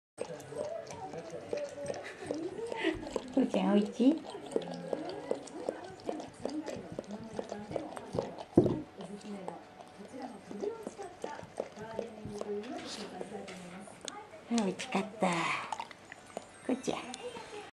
お茶が好きなゴールデンレトリバー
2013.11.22撮影　お茶が好きな我が家のゴールデン。人の飲み残しが美味しいようです（笑）。